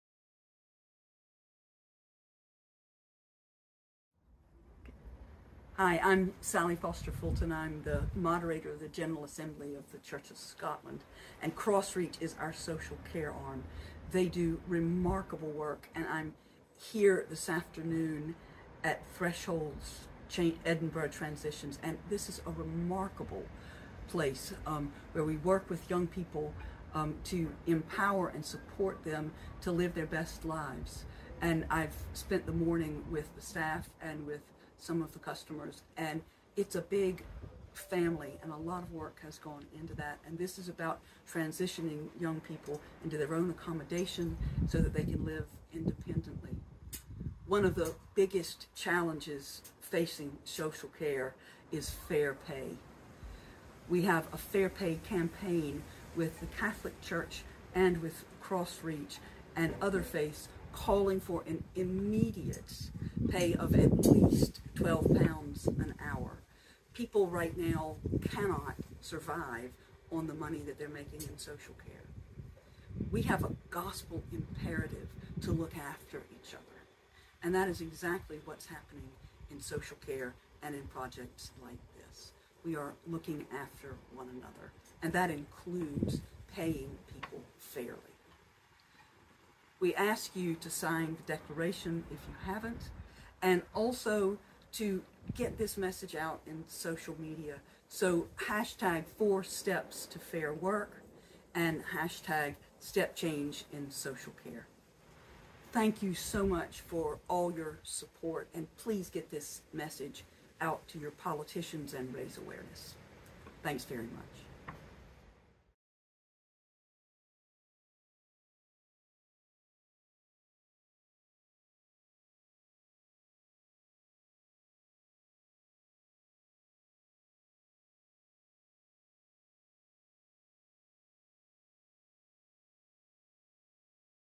Rt Rev Sally Foster-Fulton, the Moderator of the General Assembly of the Church of Scotland
Rt Rev Sally Foster-Fulton, the Moderator of the General Assembly of the Church of Scotland speaks in support of 4StepsToFairWork